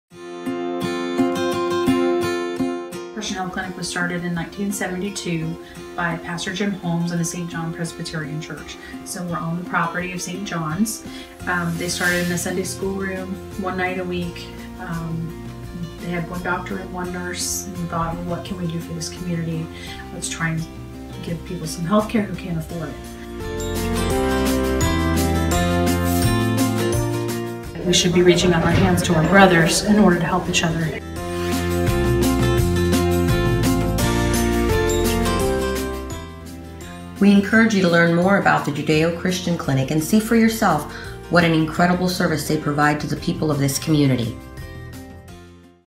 JUDEO CHRISTIAN HEALTH CLINIC
Networking Leads had the opportunity to visit the Judeo Christian Health Clinic (JCHC) in order to learn more about this amazing organization and how we could possibly offer our support.  The moment we stepped foot on the property we immediately observed how many people were benefitting from this medical service that otherwise would never have access to health care.  We did not know until visiting the clinic that it does not receive neither government or United Way funding and that it relies solely on donations and grants from individuals and organizations.  The administration and volunteers at the clinic create hope and opportunities to each person they touch.  We encourage you to take a moment to learn more about the JCHC and how you could perhaps help this organization that is doing so much for the people of this community.

Networking Leads…to help those that help!